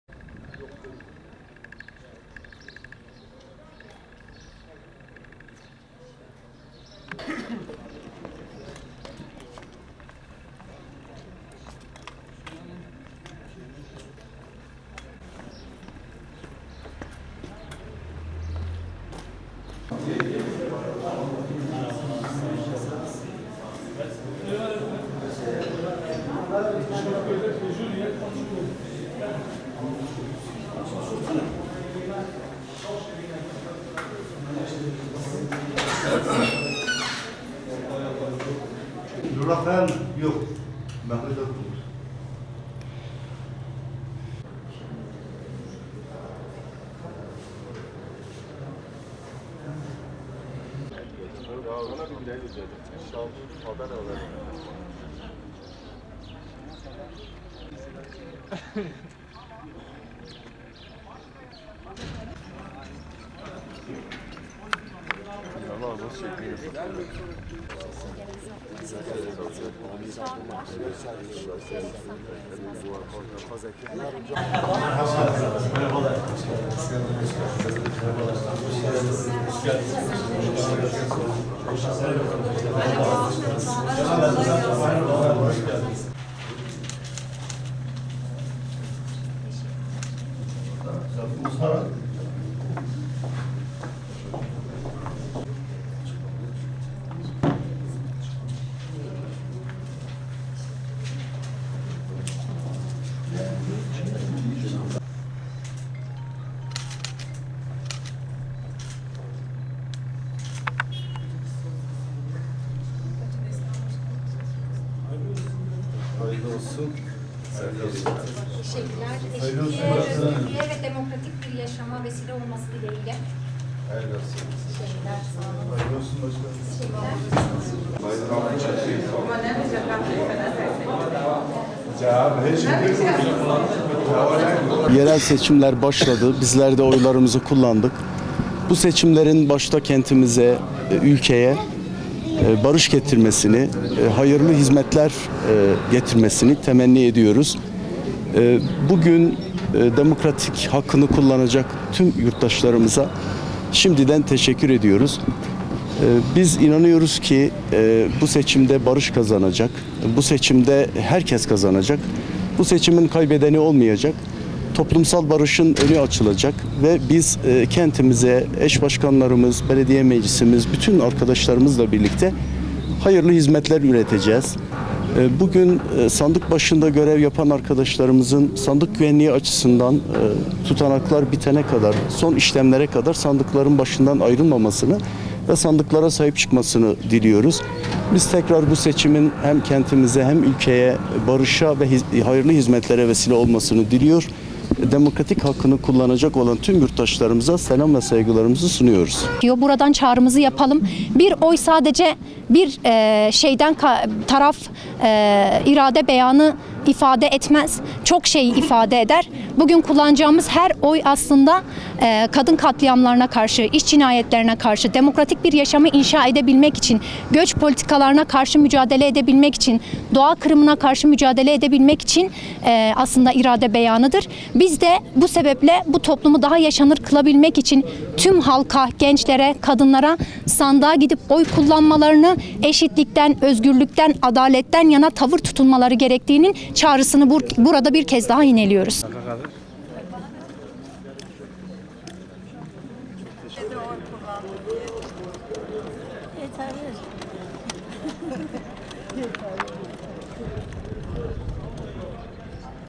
VAN DEM Parti'nin Van büyükşehir adayları oylarını kullandı (VİDEO EKLENDİ)
Behçet DALMAZ/VAN,(DHA)-HALKLARIN Eşitlik ve Demokrasi Partisi'nin (DEM Parti) Van Büyükşehir Belediye Eş Başkan Adayları Abdullah Zeydan ile Neslihan Şedal, Tuşba ilçesinde oy kullandı.
Van'da 712 bin 235 seçmen için 2 bin 462 sandık kurulurken, DEM Parti'nin Van Büyükşehir Belediye Eş Başkan Adayları Abdullah Zeydan ile Neslihan Şedal, Tuşba ilçesindeki Hürriyet İlk ve Ortaokulu'nda oy kullandı. Abdullah Zeydan, "Yerel seçimler başladı. Bizler de oylarımızı kullandık. Demokratik sistemin bir parçası olan seçimlere katılıp, oy kullanan tüm halkımıza teşekkür ederiz. Toplumsal barışın önü açılacak ve kentimize tüm arkadaşlarımız ile hayırlı hizmetlerle yürüteceğiz. Bugün sandık başında görev yapan arkadaşlarımızın sandık güvenliği açısından son işlemlere kadar sandık başından ayrılmamasını ve sandıklara sahip çıkmasını diliyoruz" dedi.
Neslihan Şedal ise "Bugün kullanacağımız her oy aslında kadın kırımına, iş cinayetlerine, göç politikalarına, doğa kırımına karşı mücadele edebilmek için irade beyanıdır. Biliyoruz ki halkın iradesinin özgürlükten, eşitlikten, demokrasiden yana sandıklara yansıyacağını bir kez daha ifade edelim. Buradan son bir çağrı ile bitirelim. Tüm halkımıza sesleniyoruz oyumuzu kullanarak irademize sahip çıkalım ve sonuçlanıncaya kadar sandıkları terk etmeyelim" diye konuştu.
Zeydan ile Şedal'a, DEM Parti İl Eş Başkanları Gülşen Kurt ile Veysi Dilekçi, eski HDP Van Milletvekili Muazzez Orhan, DBP Van İl Eş Başkanı Harun Okay  ve partililer eşlik etti.(DHA)
FOTOĞRAFLI